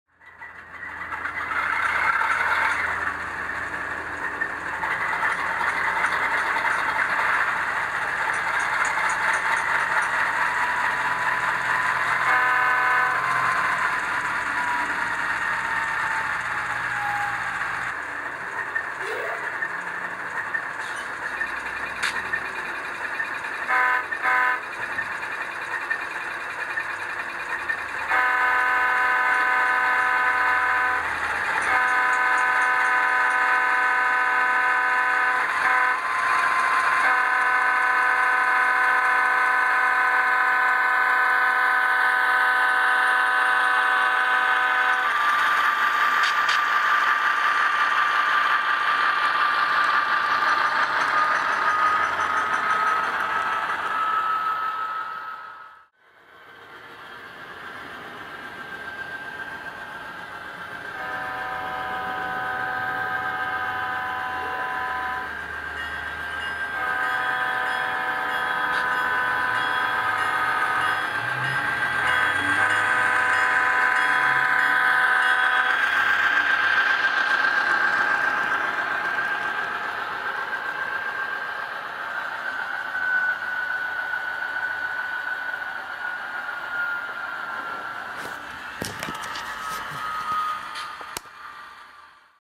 Atlas SPSF U36C with LED flashing ditch lights and tsunami sound
Custom detailed unit with flashing ditch lights. Not quite finished but it looks cool so far.